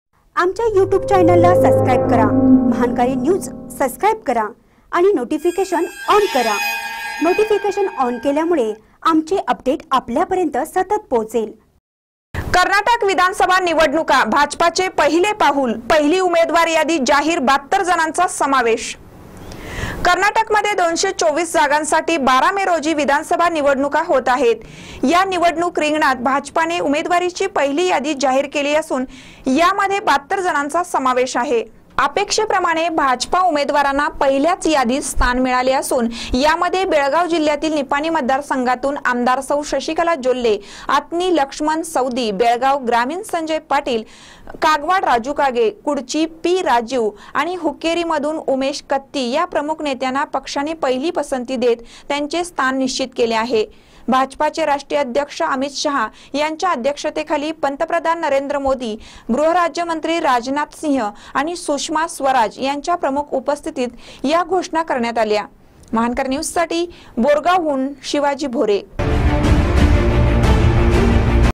कर्नाटक विधानसभा निवडणुका, भाजपचे पहिले पाऊल
कर्नाटक विधानसभा निवडणुका, भाजपचे पहिले पाऊल : पहिली उमेदवार यादी जाहीर,  ७२ जणांचा समावेश